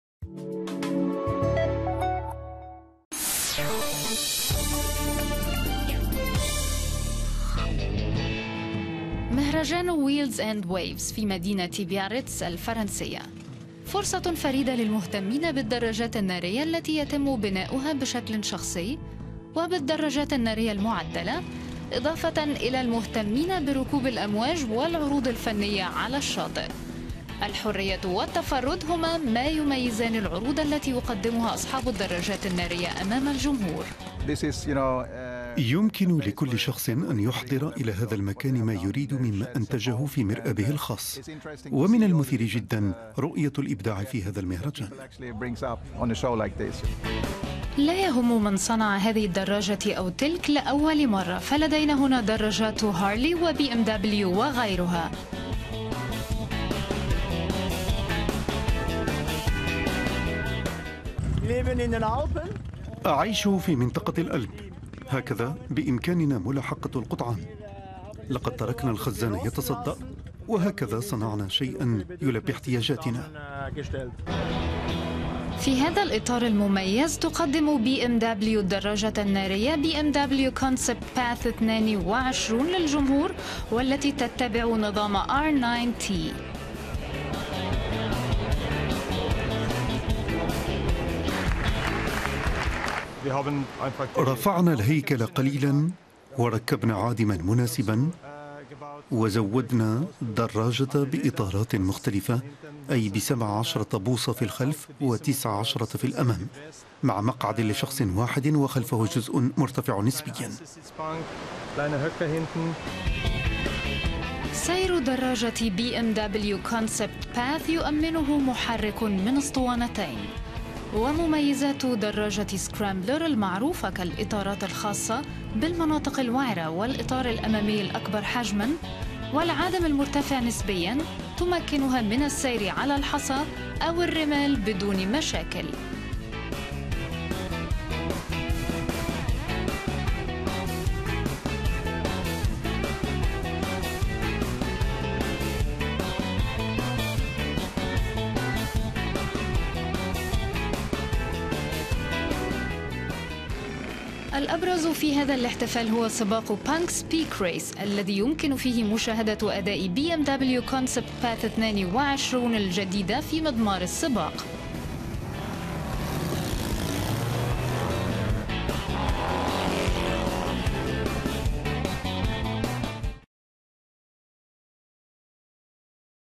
لقاء للدراجات النارية في بايرتس | عالم السرعة
يعتبر مهرجان ويل أند ويف للدراجات النارية في مدينة بايرتس الفرنسية فرصة فريدة للمهتمين بالدراجات النارية التي يتم بناؤها بشكل شخصي والدراجات النارية المعدلة إضافة إلى ركوب الأمواج والعروض الفنية على الشاطئ.